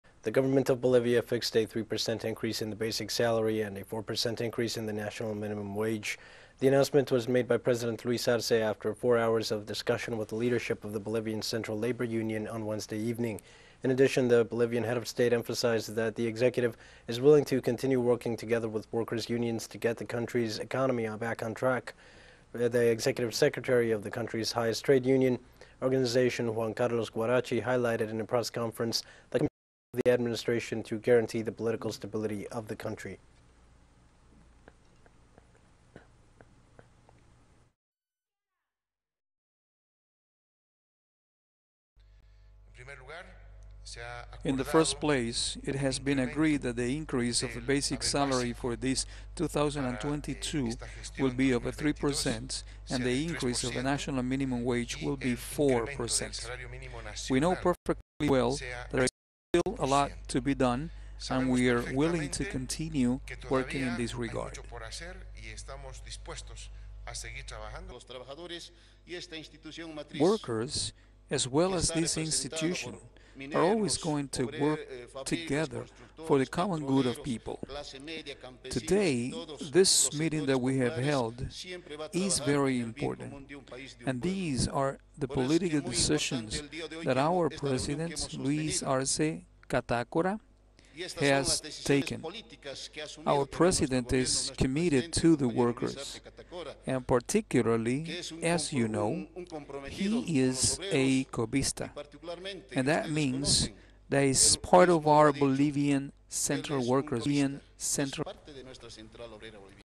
Bolivia: President Luis Arce materializes salary increase for the entire nation
The Government of Bolivia fixed a three percent increase in the basic salary and a four percent increase in the national minimum wage.teleSUR